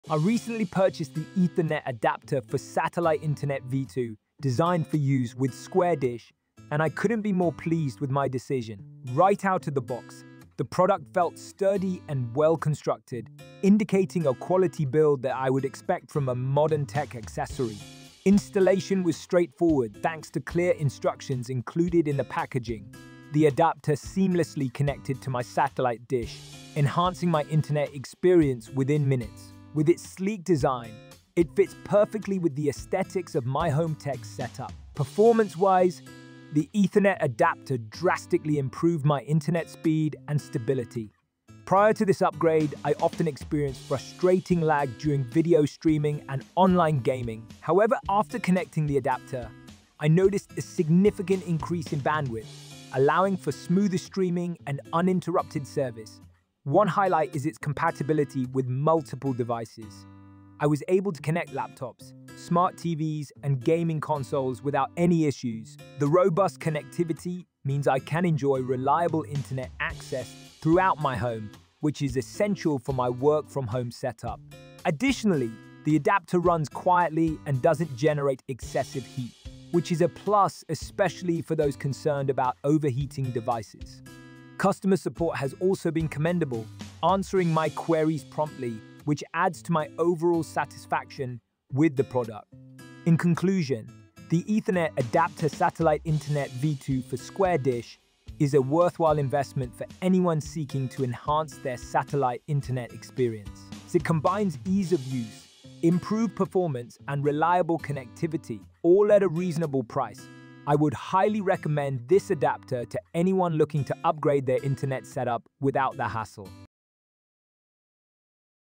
Ethernet Adapter Satellite Internet V2 New for Square Dish New in Box Review
I recently purchased the Ethernet Adapter for Satellite Internet V2, designed for use with Square Dish, and I couldn’t be more pleased with my decision. Right out of the box, the product felt sturdy and well-constructed, indicating a quality build that I would expect from a modern tech accessory.

Installation was straightforward, thanks to clear instructions included in the packaging. The adapter seamlessly connected to my satellite dish, enhancing my internet experience within minutes. With its sleek design, it fits perfectly with the aesthetics of my home tech setup.

Performance-wise, the Ethernet adapter drastically improved my internet speed and stability. Prior to this upgrade, I often experienced frustrating lag during video streaming and online gaming. However, after connecting the adapter, I noticed a significant increase in bandwidth, allowing for smoother streaming and uninterrupted service.

One highlight is its compatibility with multiple devices. I was able to connect laptops, smart TVs, and gaming consoles without any issues. The robust connectivity means I can enjoy reliable internet access throughout my home, which is essential for my work-from-home setup.

Additionally, the adapter runs quietly and doesn't generate excessive heat, which is a plus, especially for those concerned about overheating devices. Customer support has also been commendable, answering my queries promptly, which adds to my overall satisfaction with the product.

In conclusion, the Ethernet Adapter Satellite Internet V2 for Square Dish is a worthwhile investment for anyone seeking to enhance their satellite internet experience. It combines ease of use, improved performance, and reliable connectivity—all at a reasonable price. I would highly recommend this adapter to anyone looking to upgrade their internet setup without the hassle.